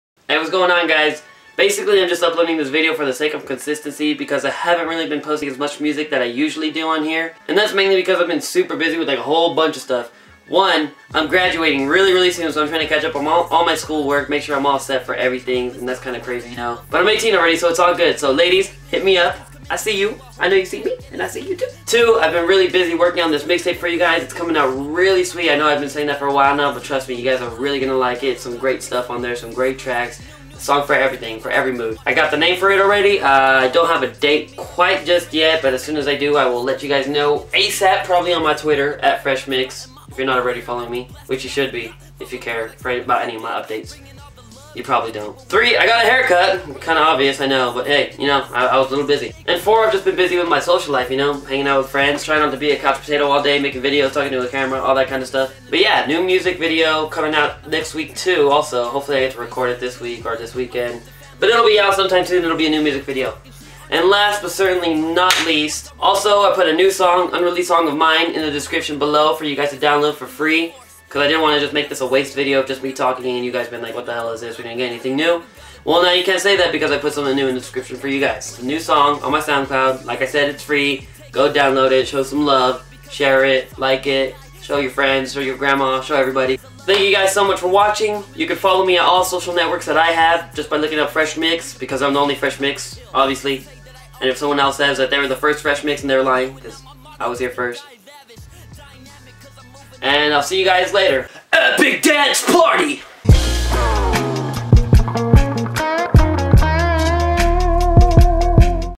NEW TRACK + UPDATES + SUPER FUN DANCE TIME
Sorry I've been away from youtube a little longer than I usually am. I've been super busy with a lot of music things and I thank you guys for sticking with me.